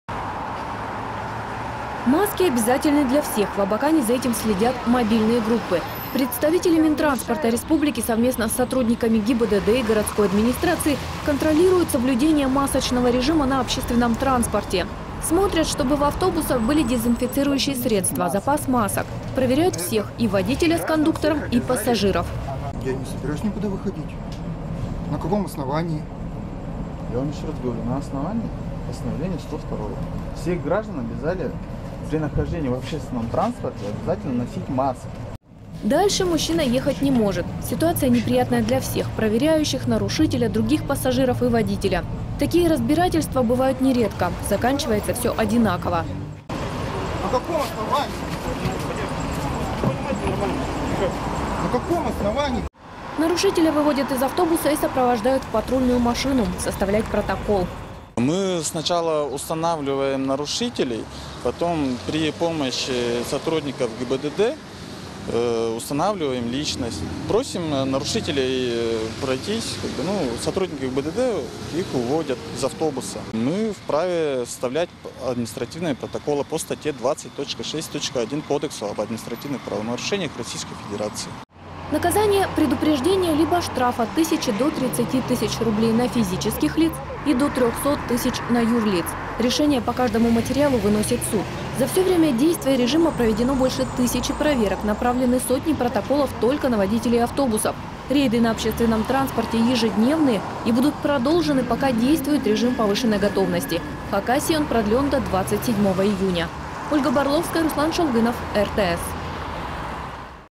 Общественный транспорт в Хакасии проверяют на соблюдение масочного режима.
Маски обязательны для всех. В Абакане за этим следят мобильные группы. Представители Минтранса Хакасии совместно с сотрудниками ГИБДД и городской администрации контролируют соблюдение масочного режима на общественном транспорте. Смотрят, чтобы в автобусах были дезинфицирующие средства, запас масок. Проверяют всех - и водителя с кондуктором, и пассажиров.